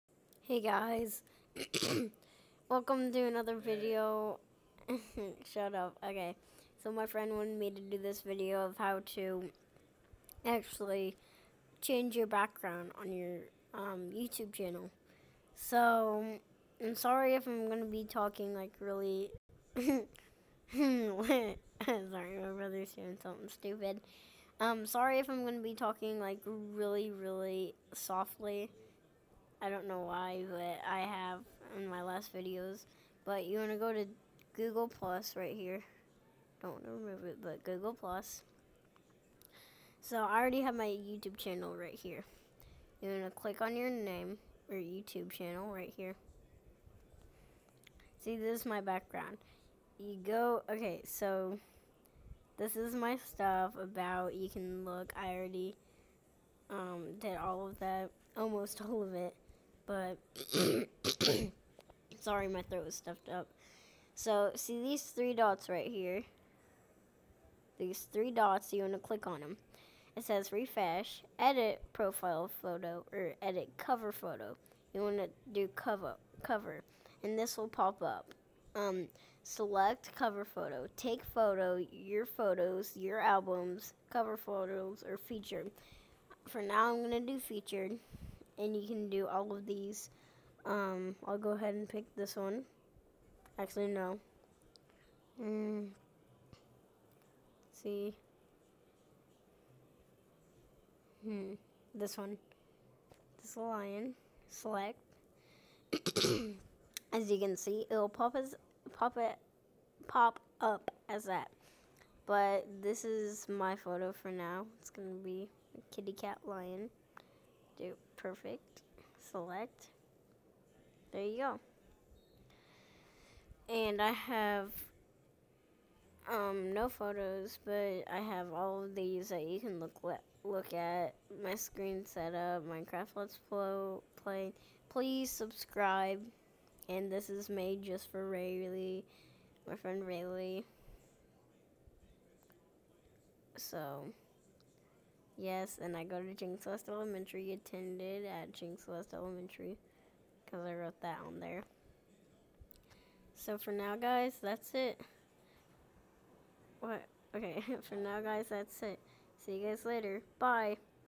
How to change your back ground photo | made for ra
My best friend asked me to make a background video because she doesn't know how to make a background photo